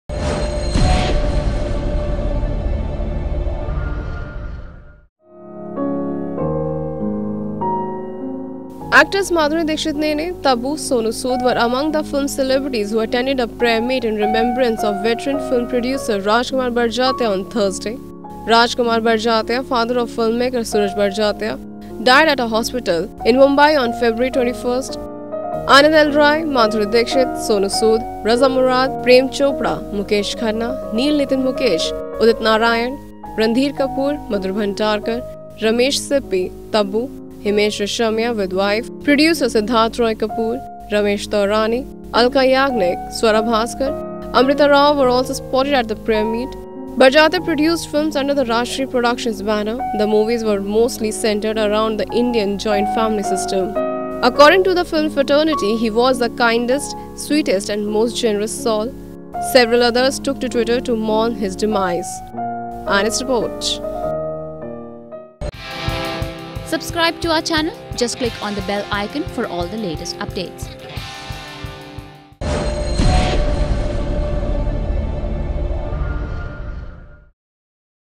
Madhuri, Tabu & others attend Raj Kumar Barjatya's prayer meet
Actors Madhuri Dixit-Nene, Tabu, Sonu Sood were among the film celebrities who attended a prayer meet in remembrance of veteran film producer Raj Kumar Barjatya, on Thursday. Raj Kumar Barjatya, father of filmmaker Sooraj Barjatya, died at a hospital here on February 21. Anand L Rai, Madhuri Dixit, Sonu Sood, Raza Murad, Prem Chopra, Mukesh Khanna, Neil Nitin Mukesh, Udit Narayan, Randhir Kpaoor, Madhur Bhandarkar, Ramesh Sippy, Tabu, Himesh Reshammiyan with wife, Sidharth Roy Kapur, Ramesh Taurani, Alka Yagnik, Swara Bhasker, Amrita Rao were also spotted at the prayer meet.